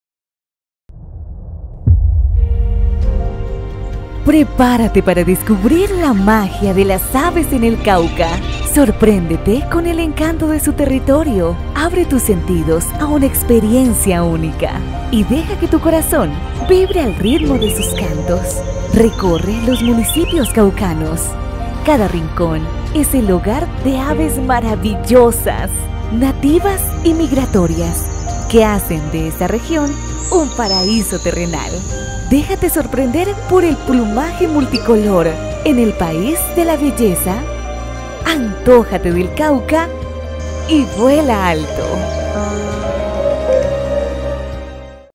🥳¡Pajareros en el Cauca, llegó el día! 🥽Este sábado 11 de mayo vamos a disfrutar el GlobalBigDay
🥳¡Pajareros en el Cauca, llegó el día! 🥽Este sábado 11 de mayo vamos a disfrutar el GlobalBigDay con alegría y amor a las aves.🐦Utiliza la APP eBird o ingresa a para registrar  las especies avistadas.

➡️Visita el perfil de @[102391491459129|Asociación Caucana de Observadores de Aves] y consulta la agenda programada en los municipios del Cauca que se sumarán a este gran día, ¡participa junto a tu grupo de amigos y aficcionados!🪶🦅🦉